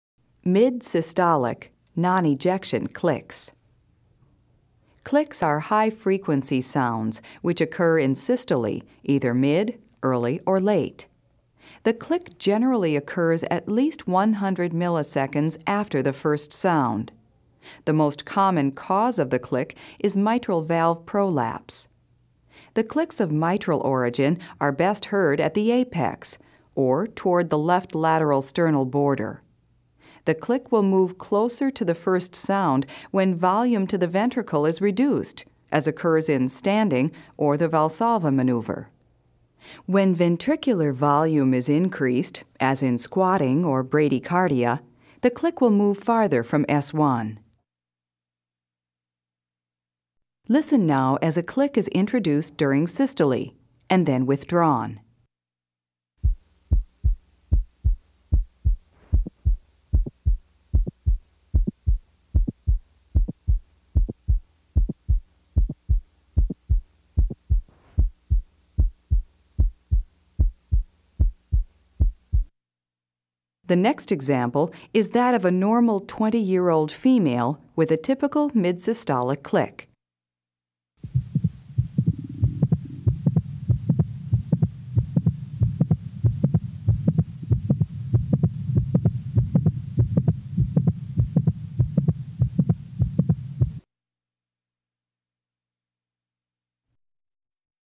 17. Mid-systolic Non-ejection Clicks - Heart Sounds Collection
This series contains a collection of various heart and lung sounds as heard on auscultation and a basic explanation of where to hear it and in which conditions it occurs.